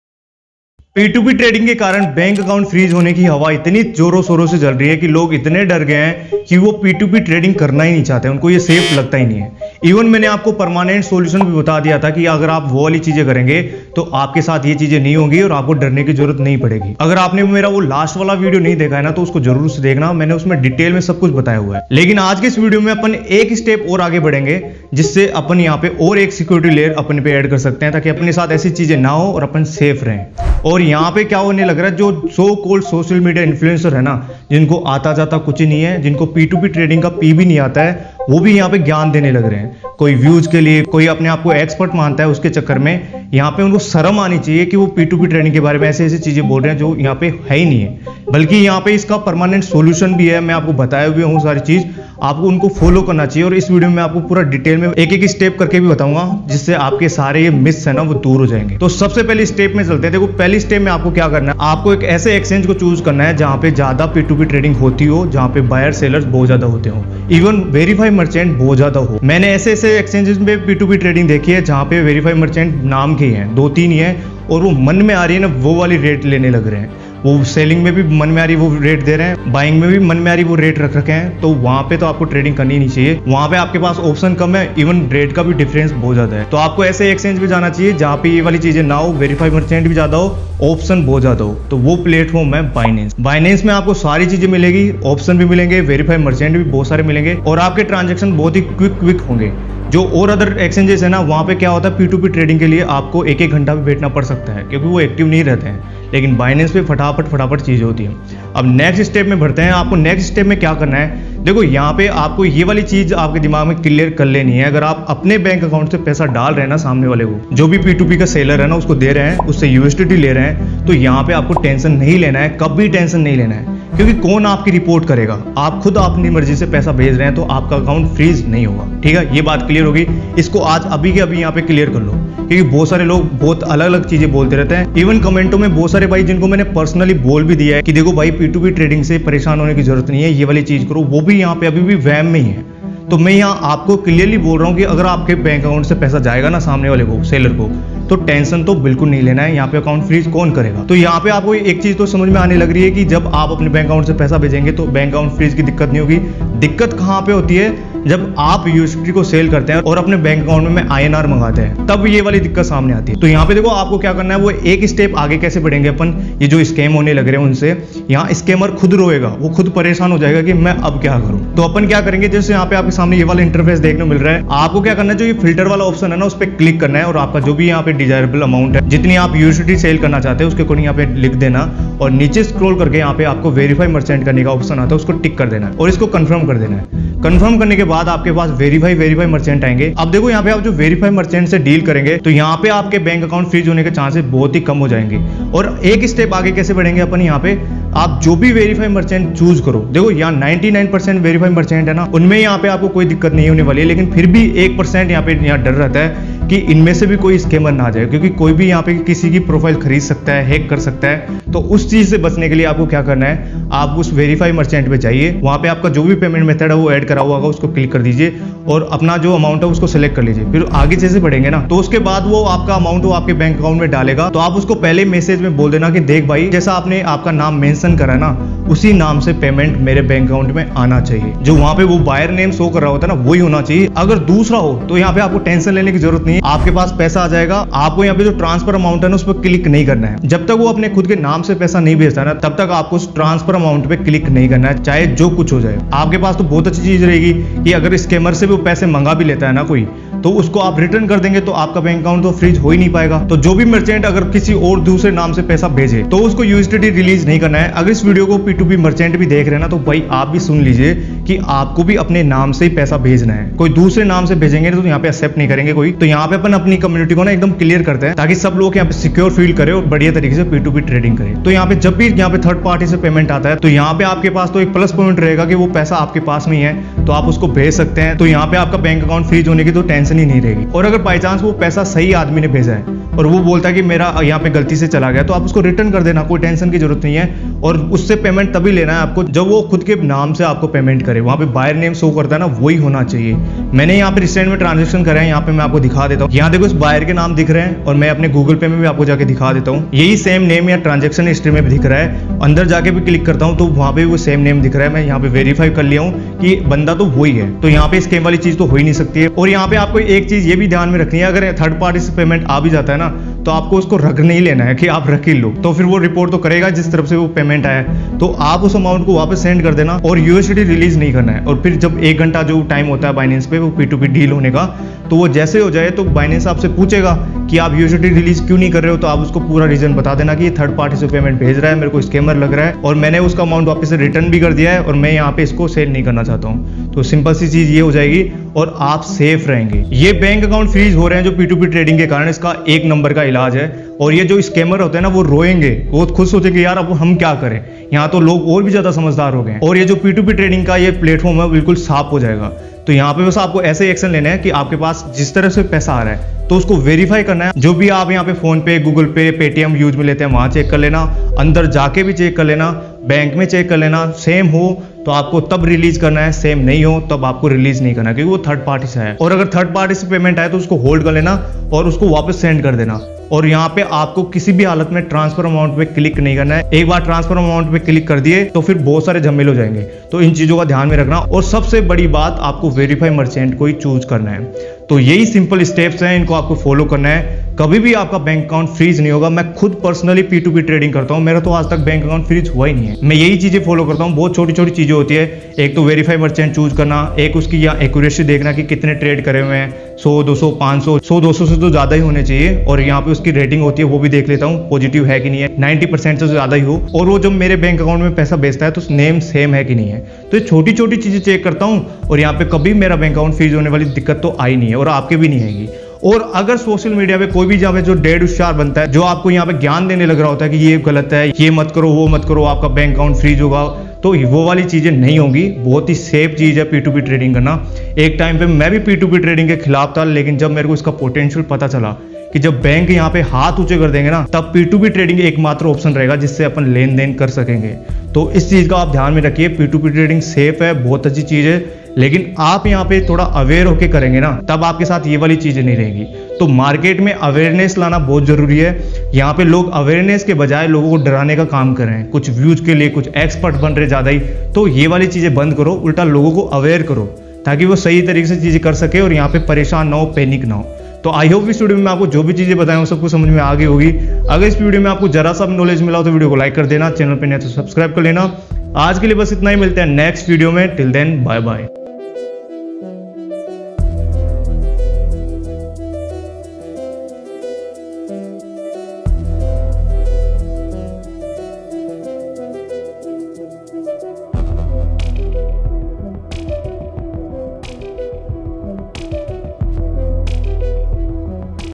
🔥 BANK Account Freeze No More: ADVANCE Solution for Successful Binance P2P Trading 🚀
Anurag Yaduvanshi and any person associated with this channel accepts no liability for any kind of content uploaded on this channel or responsibility for any